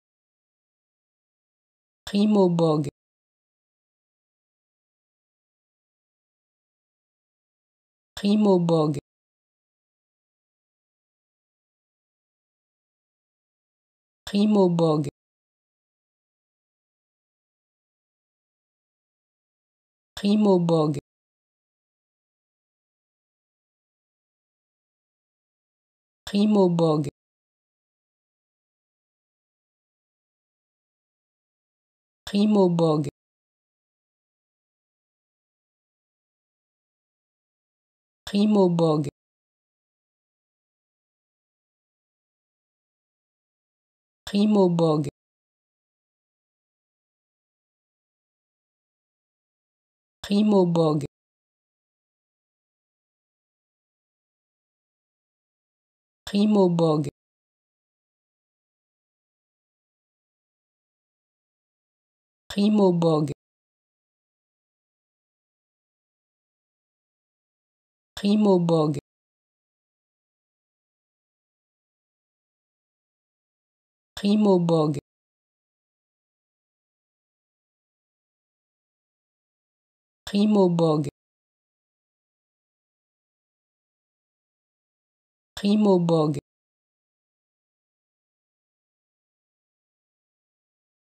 How to Pronounce ''Prime aux bogues'' (Bug Bounty) Correctly in French
Learn how to say and properly pronounce ''Prime aux bogues'' in French with this free pronunciation tutorial.